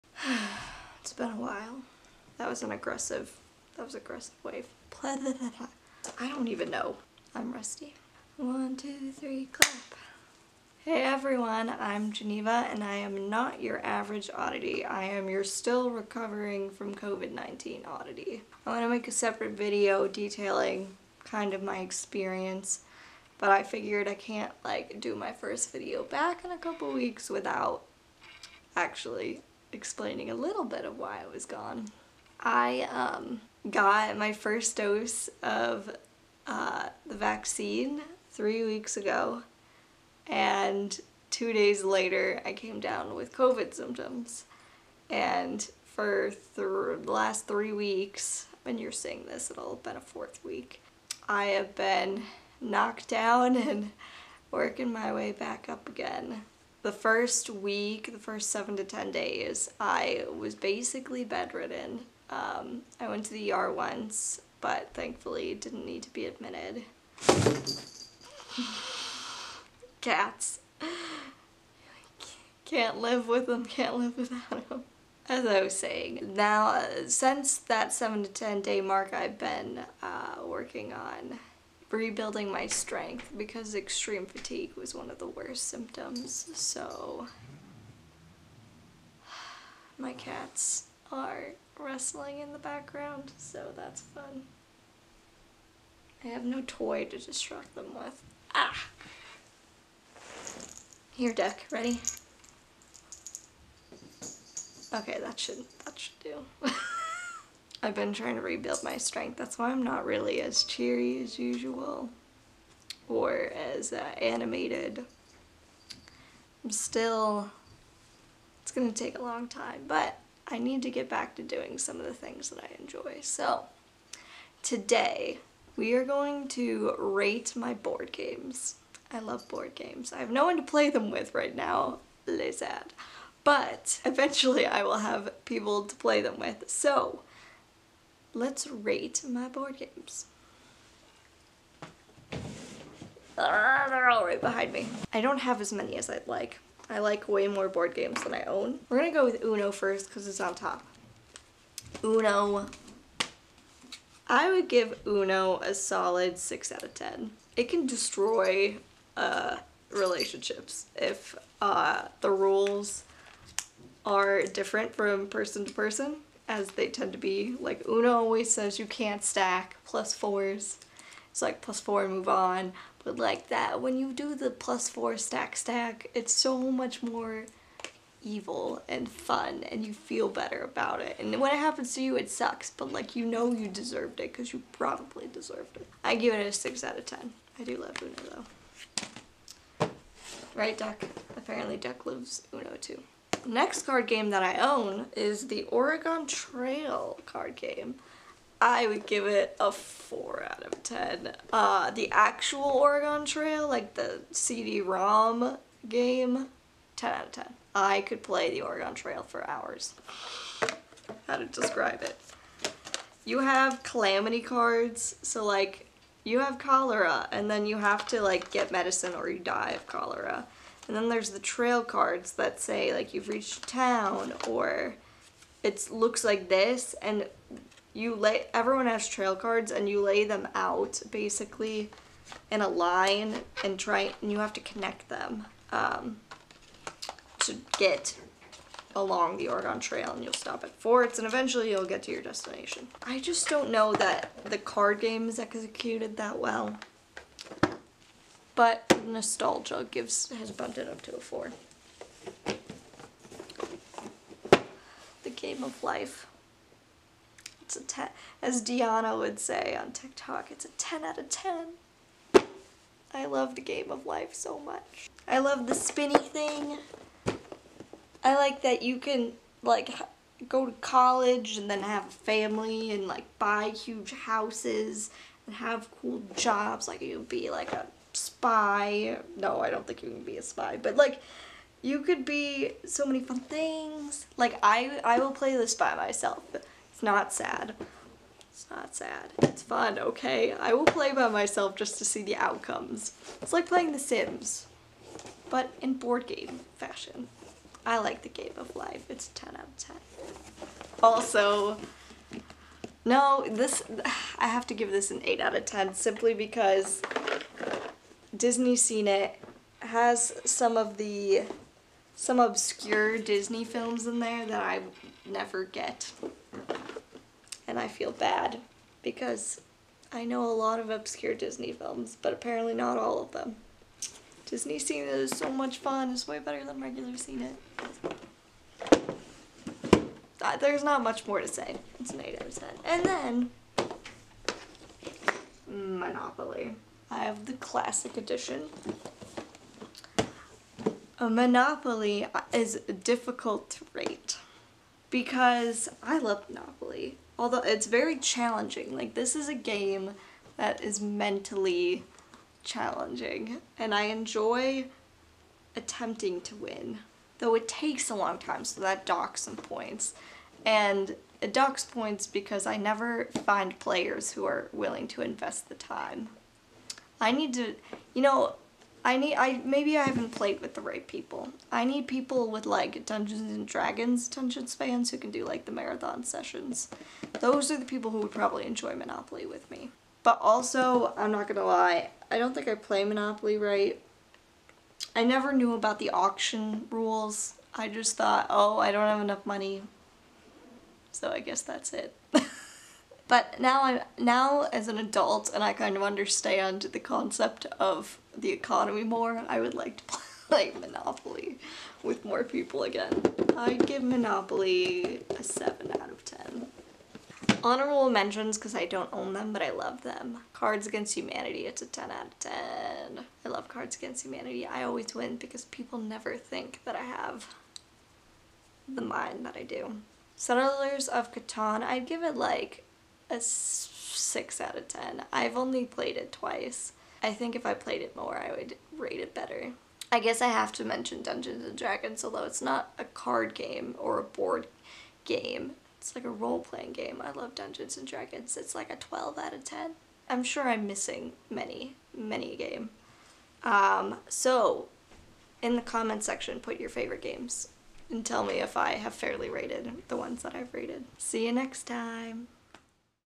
Rating Board Games! (Also, I'm Back!)
Hey ya'll, I'm Geneva, and I am not your average oddity. I took a short 2 week hiatus after coming down with Covid-19. A video explaining more will come out later, but today, we are rating board games. Was I fair? Are there any games I should rate?